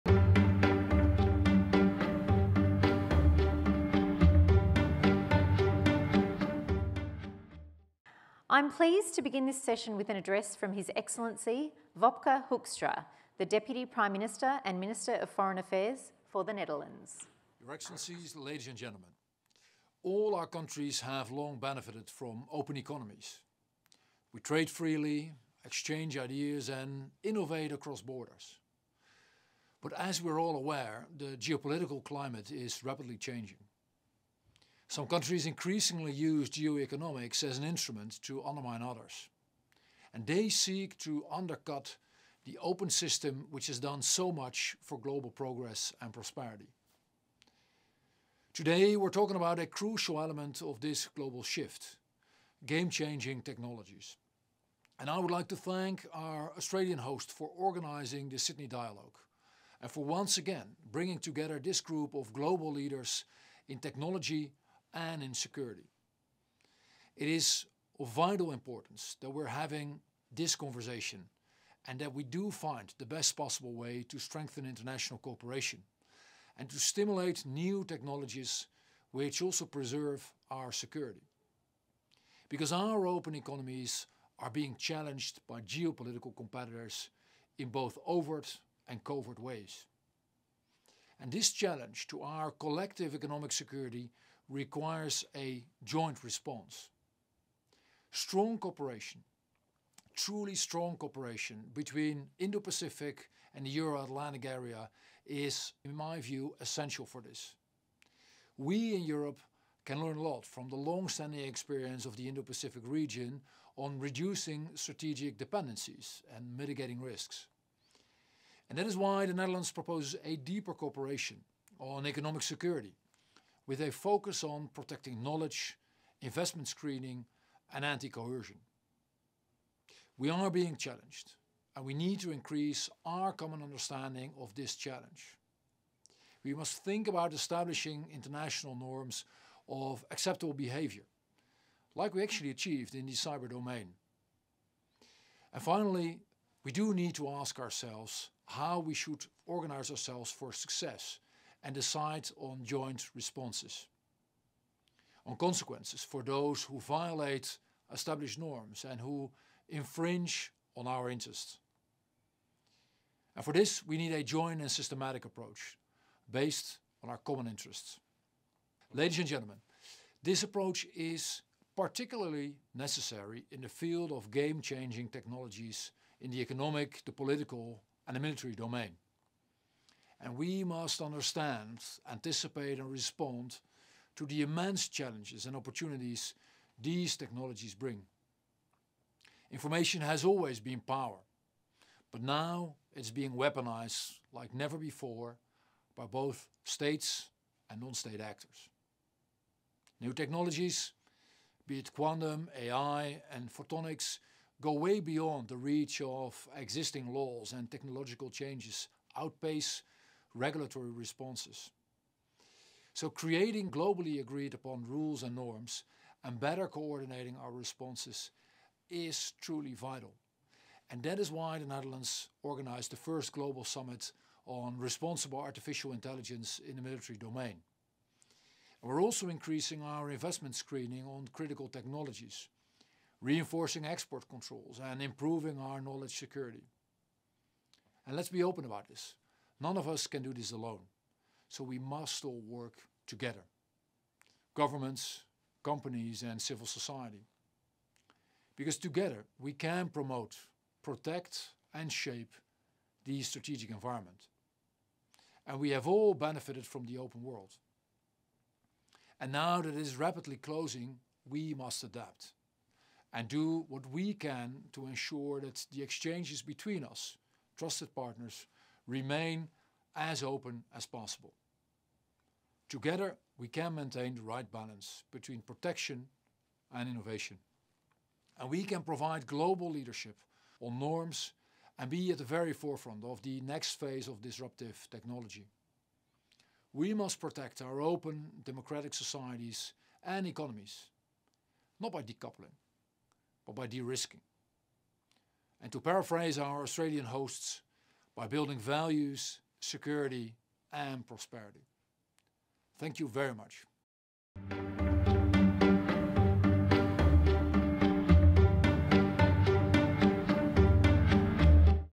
The Netherlands Government Address: Wopke Hoekstra
This keynote address from the former Deputy Prime Minister of The Netherlands Wopke Hoekstra focused on the rapidly changing geopolitical climate. The speech highlighted how open economies are being challenged by geopolitical competitors in overt and covert ways, and called on like-minded partners to cooperate on setting globally agreed upon rules and norms. The former Deputy Prime Minister also spoke about the lessons Europe can learn from the Indo-Pacific region on reducing strategic dependencies and mitigating risks.

Speakers in this session included: H.E. Wopke Hoekstra, Former Deputy Prime Minister of The Netherlands and Minister of Foreign Affairs.

Master of Ceremonies: Nina Walsh.